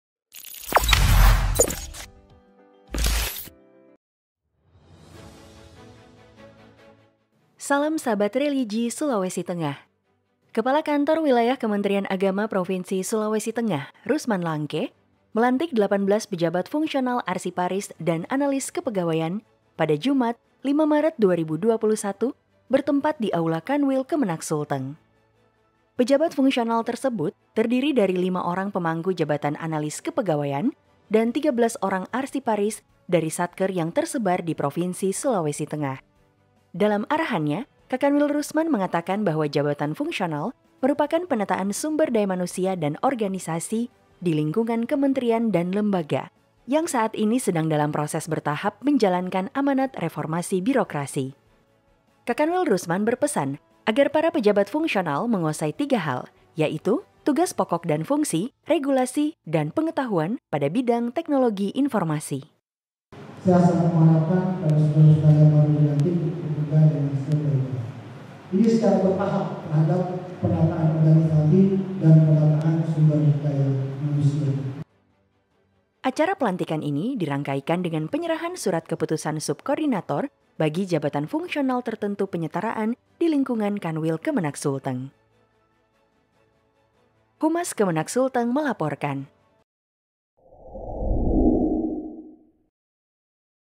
Kakanwil Kemenag Sulteng Lantik 18 Pejabat Fungsional
Pelantikan Pejabat Fungsional Arsiparis dan Analis Kepegawaian Se-Provinsi Sulawesi Tengah, Jum'at 5 Maret 2021